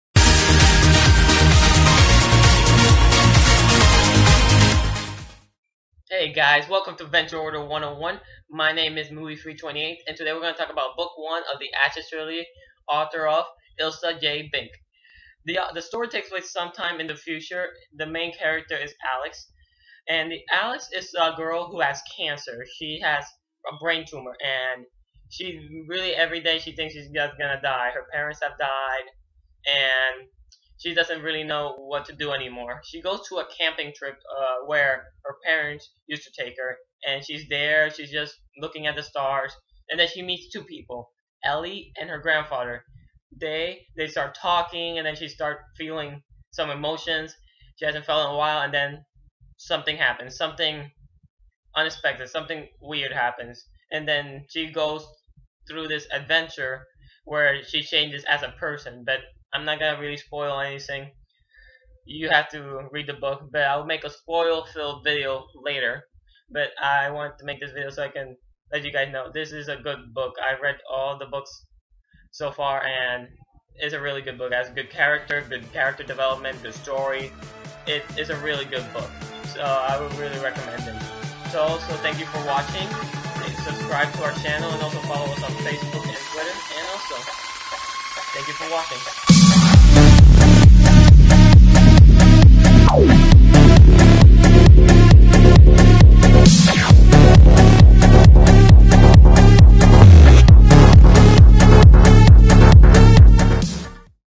【Book Review】The Ashes Trilogy: Ashes
In this video, Moviefreak28 reviews The Ashes Trilogy, Book I, Ashes by Ilsa J. Bick.